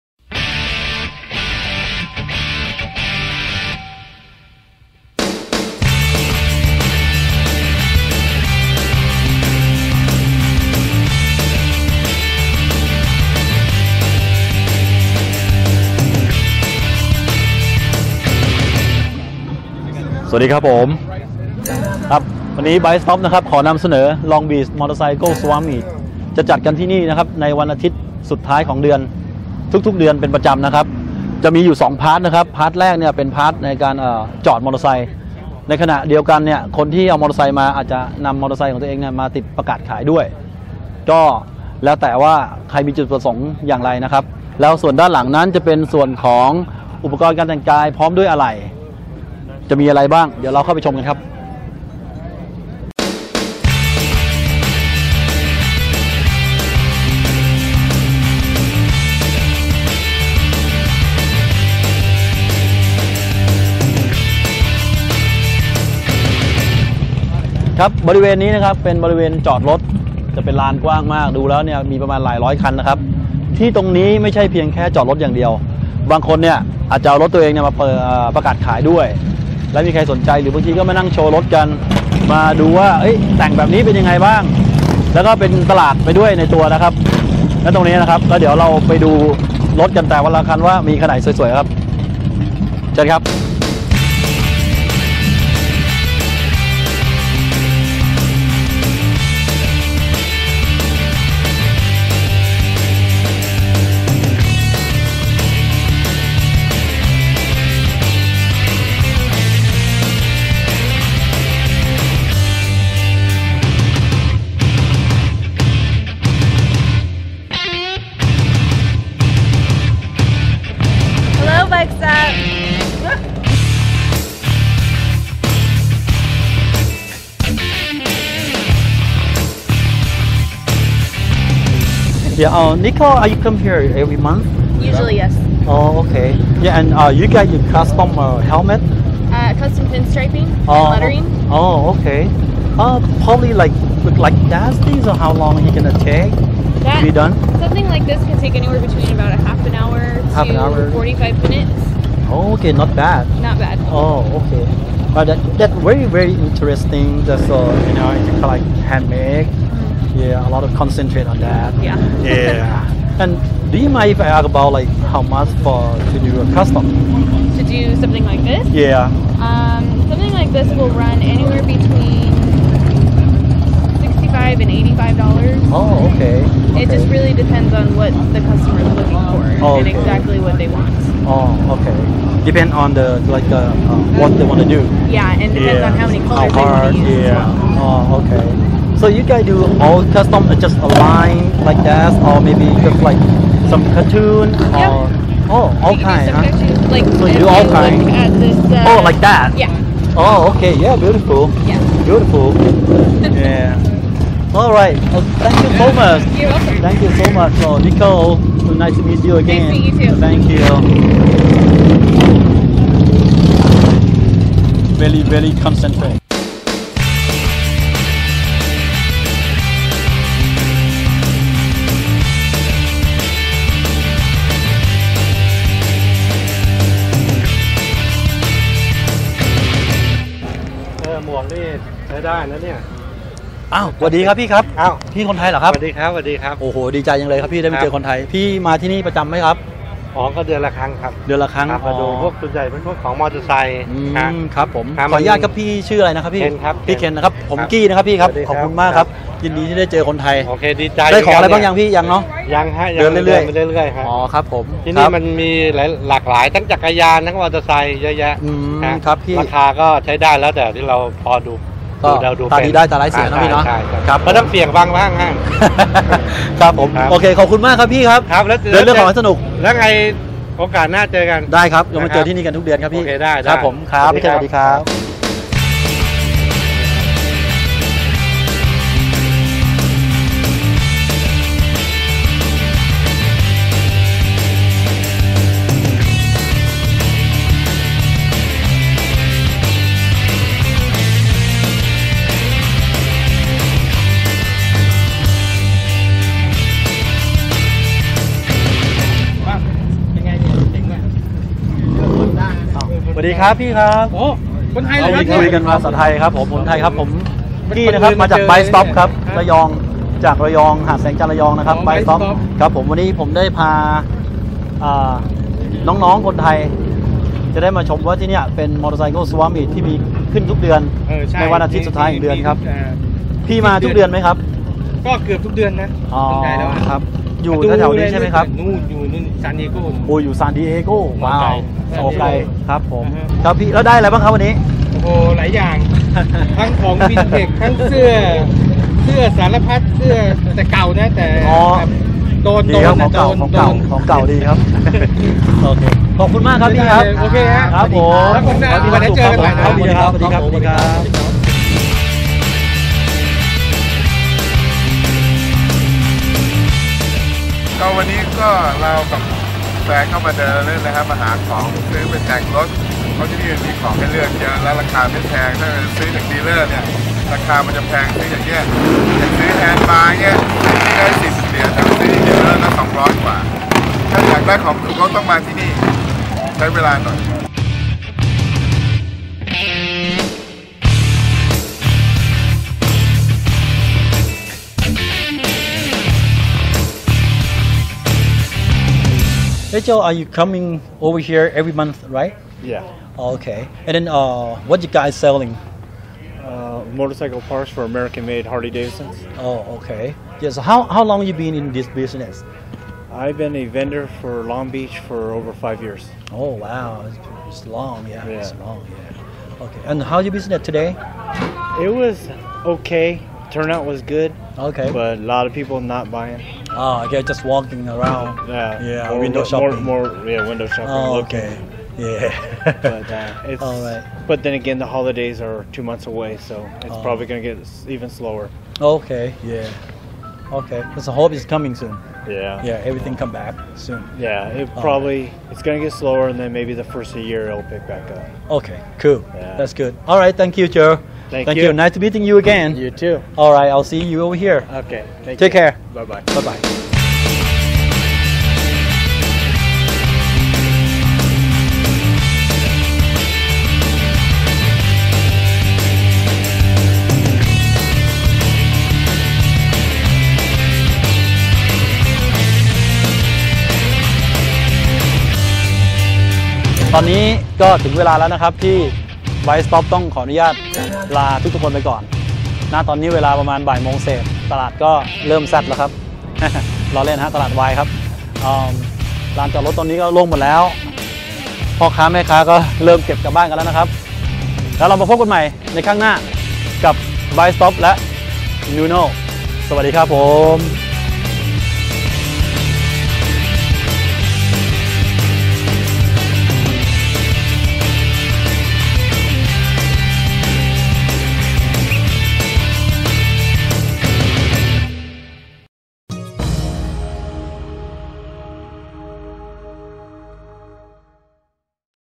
Bike Stop & NUNO @ Socalcycle Swapmeet
The So-Cal Cycle Swap Meet is the largest monthly gathering of riders in the USA. Held every month at Long Beach Veteran’s Stadium the So-Cal Cycle Swap Meet is an “All Brands” event with over 500 Vendors and over 1000 Bikes for show and sale. Riders and Collectors from all ends of the bike hobby, including Harley, Cruisers, Choppers, Vintage, British, Race, Off-Road, Japanese, Euro, and Bicycles get together to buy, sell, trade, show off, and talk about new, used, and vintage parts, bikes, and accessories. From professional shops and manufacturers liquidating excess inventory to the hobbyist selling off leftovers from his latest project you can find smokin’ deals on hard to find parts and accessories. Ride out and park your bike in the Bike Corral or get a vendor space and unload those old parts from your garage.